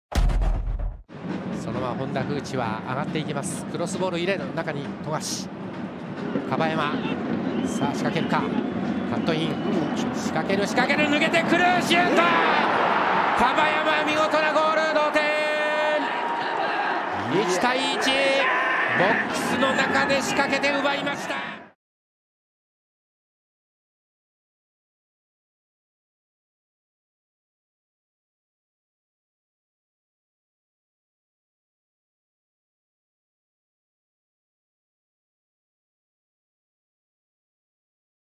2023年2・3月度のＪ１月間ベストゴールは樺山 諒乃介（鳥栖）に決定！
２０２３明治安田生命Ｊ１リーグ 2・3月度の月間ベストゴールの動画です。
第2節 2/25（土）ガンバ大阪vsサガン鳥栖の64分に生まれた鳥栖の樺山 諒乃介選手によるゴールに決定しました。